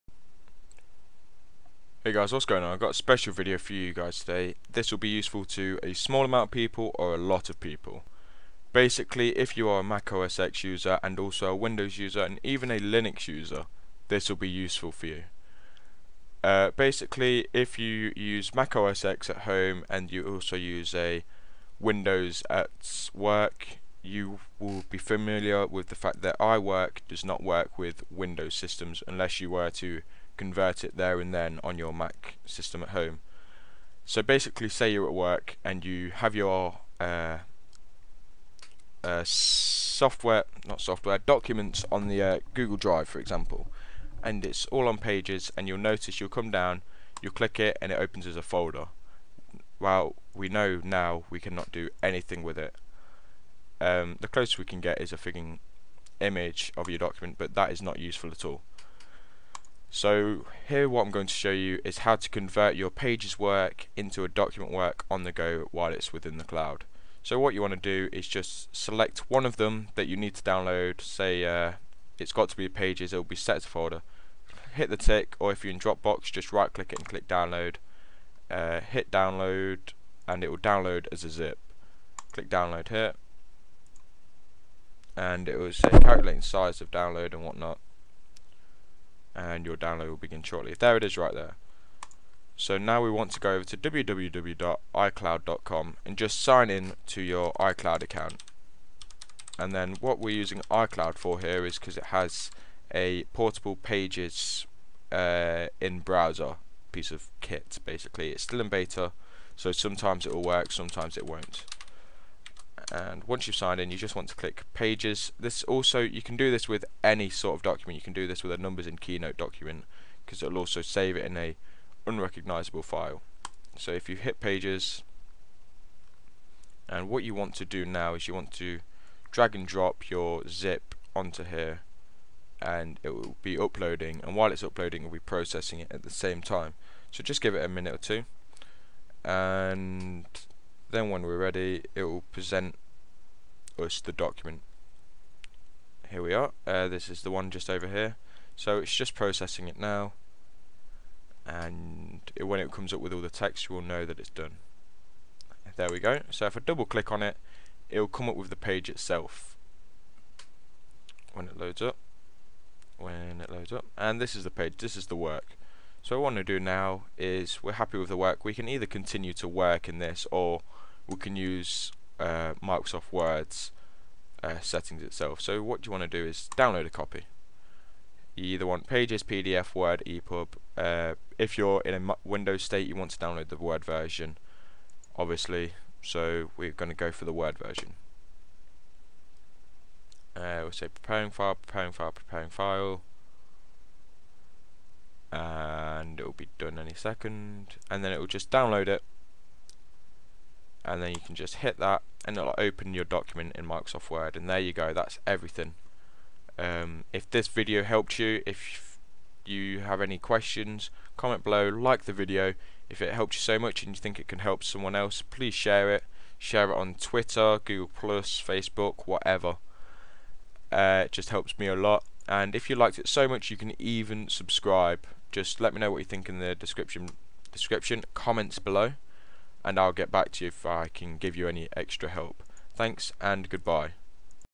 How to convert Pages and other Apple files on Windows!
READ DESCRIPTION

Hey guys, another video here going over the frustration of having to open a pages document which has been saved in google drive. all you need is your drive account, and your original Apple ID!

Feedback much appreciated!